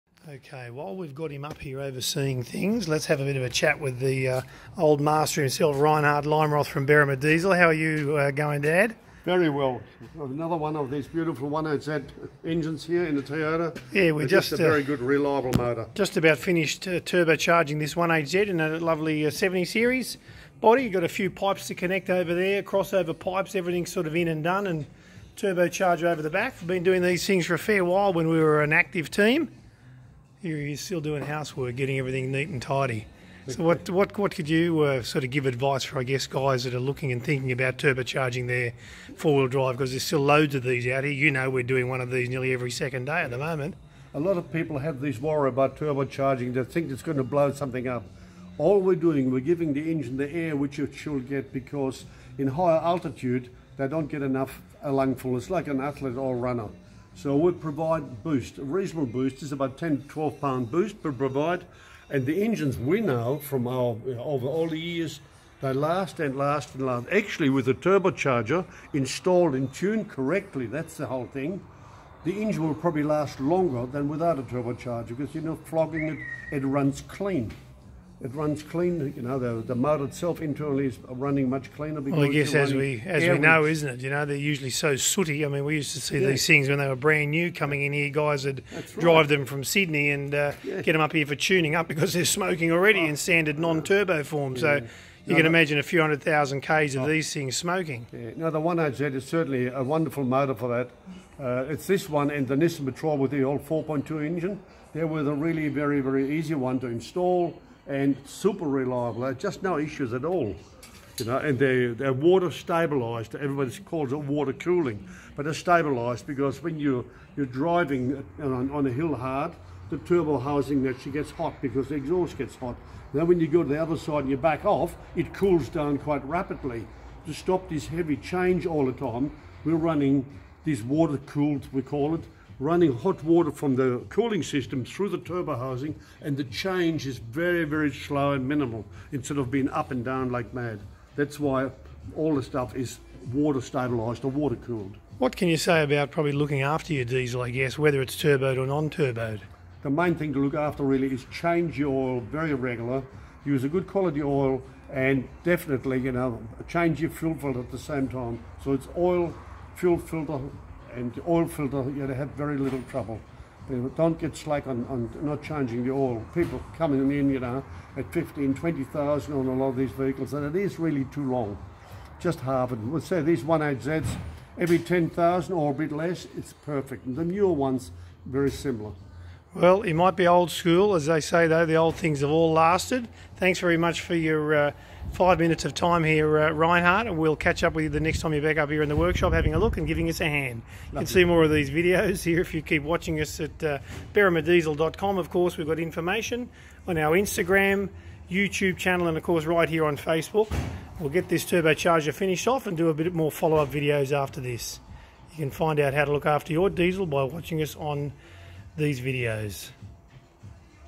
Berrima Diesels own Bosch Guru explains why he loves to Turbo charge Diesels
Here's Bosch Guru and owner of Berrima Diesel Reinhard Leimroth explaining why he loves to Turbo charge Diesels like the TD42 and 1HZ.

Or PMessage us right here